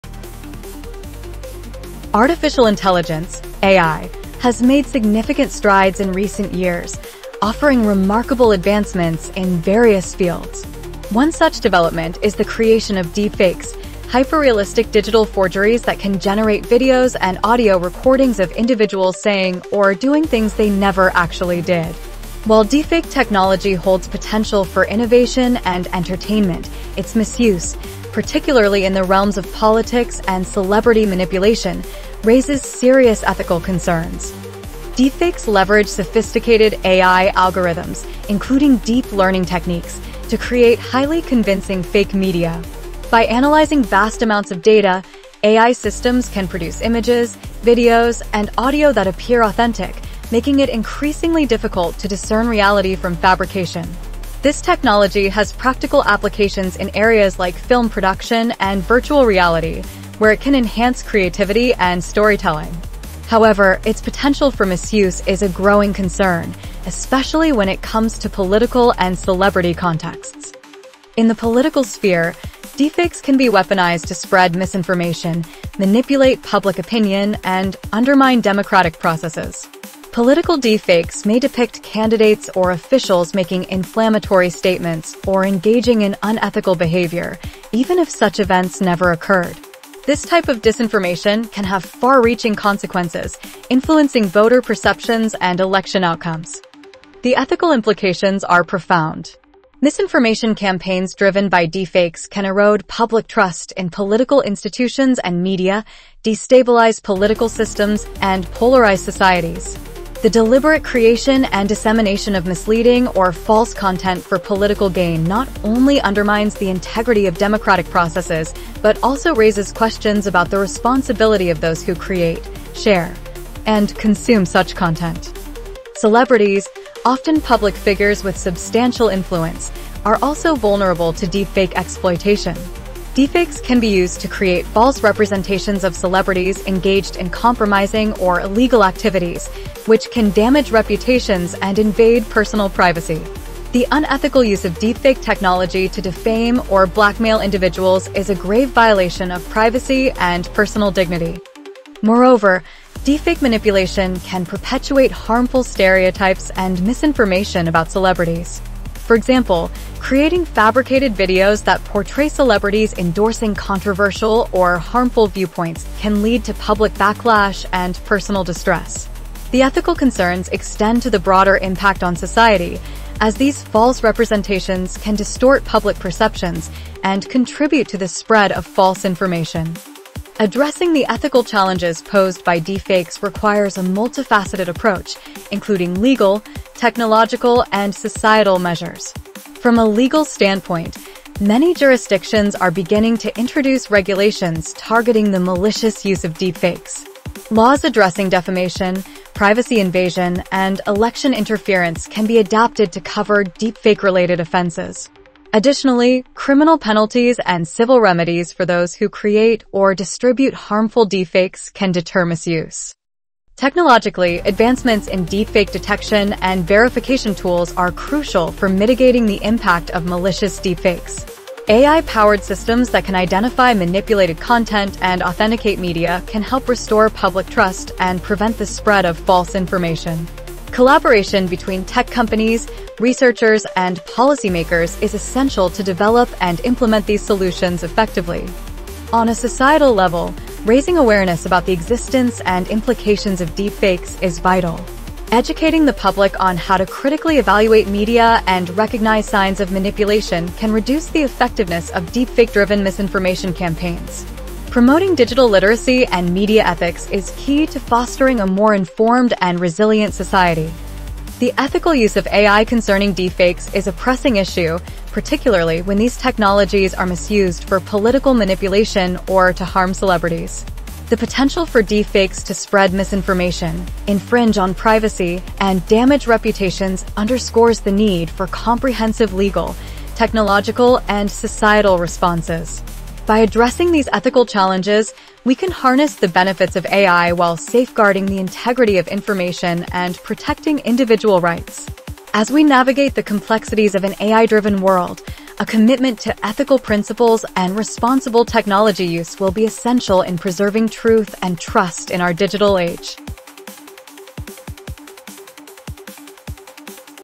The Ethical Impact of AI on Privacy and Autonomy/A.I. Ethics & Morals Today & Tomorrow
The Ethical Impact of AI on Privacy and Autonomy

In this video, we delve into the complex ethical landscape surrounding artificial intelligence, focusing on the impact of deepfake technology on politics and celebrity manipulation.

🤖 Key Topics Covered:

Understanding Deepfakes: What they are and how they work.
Political Implications: How deepfakes can be used to spread misinformation, manipulate public opinion, and undermine democratic processes.
Celebrity Privacy: The ethical issues related to the creation of fake media involving celebrities, including privacy violations and reputation damage.
Legal and Ethical Responses: Exploring current legal frameworks and technological solutions designed to combat malicious deepfake use.
Societal Impact: The importance of digital literacy and public awareness in mitigating the effects of deepfakes.

🔍 Why This Matters: As AI technology advances, understanding its ethical implications becomes crucial. Deepfakes, with their potential to deceive and manipulate, present significant challenges. This video provides insights into how we can address these issues responsibly, ensuring that technology serves the greater good without compromising truth and trust.

Read by MS Word
Written by ChatGPT and Ald the Elder
Revised by Grammarly Intuitive A.I.
Art by PixAI
Edited by Ald the Elder